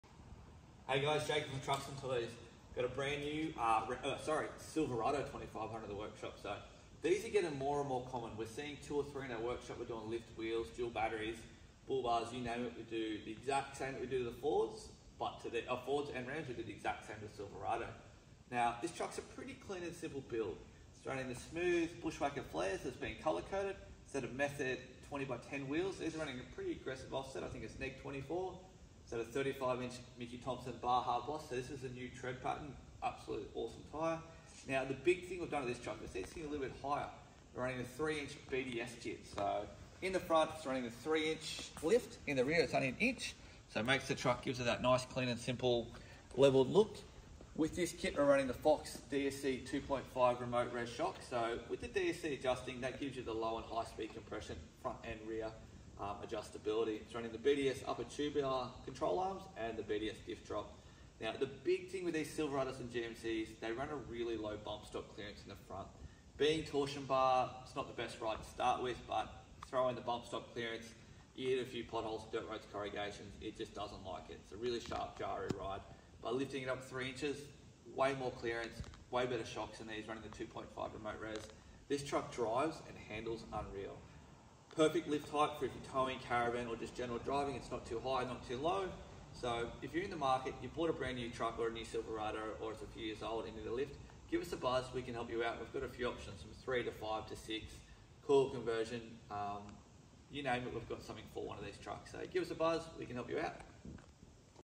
2024 Silverado 2500 3500 lifted BDS SUSPENSION WITH FOX DSC 2.5 shocks AUSTRALIA ￼METHOD RACEWHEEL
Brand-new Silverado 24 model with 3inch BDS suspension kit,  20inch Method Race Wheel,  smooth flares and 35 inch Mickey Thompson tyres ￼